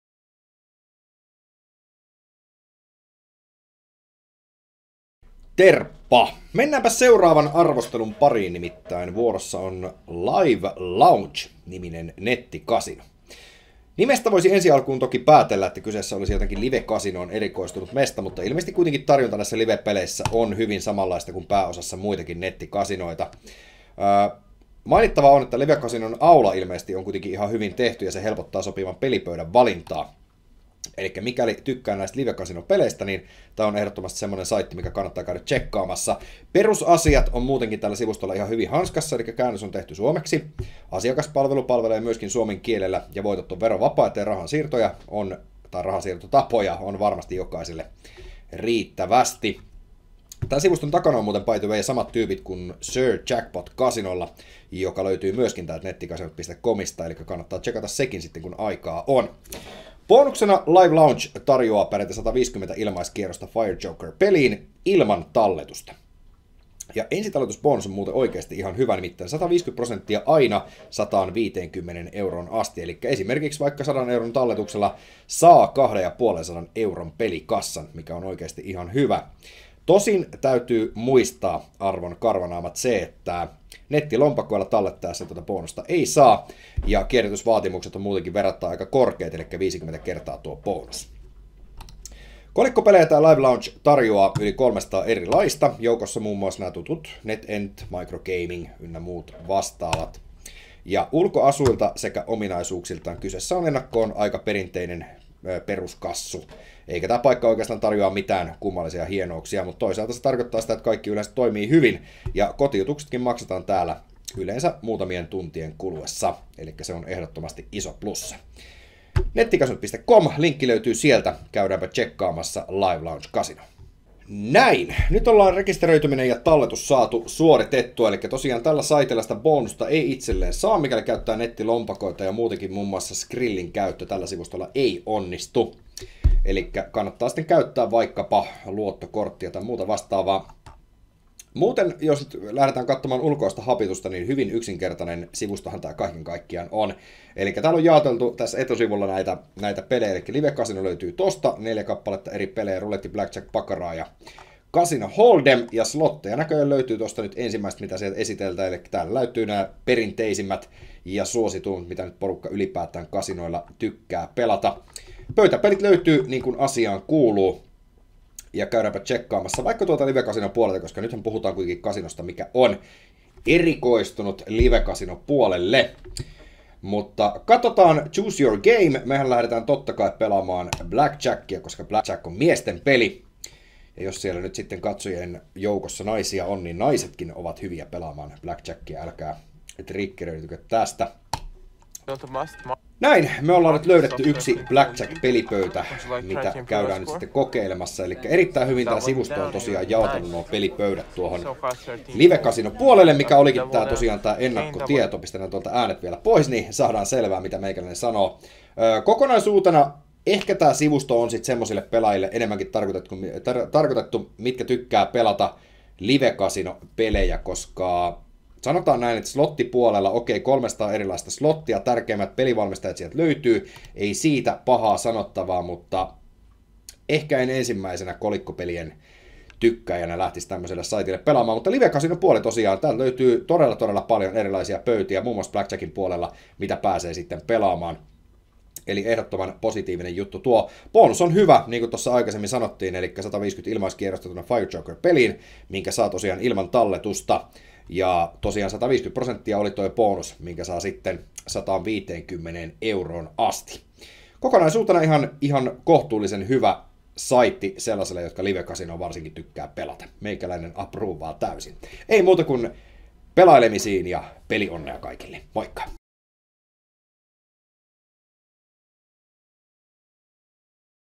Live Lounge Casino - Esittely, Bonus & Ilmaiskierrokset
Live Lounge Casino on etenkin livepelien ystäville mainiosti sopiva suomenkielinen nettikasino. Uusille pelaajille annetaan huimat 150 ilmaiskierrosta ilman talletusta ja ensitalletusbonus on muotoa 150%/150€ eli satasen talletuksella saa mukavan 250€ pelikassan.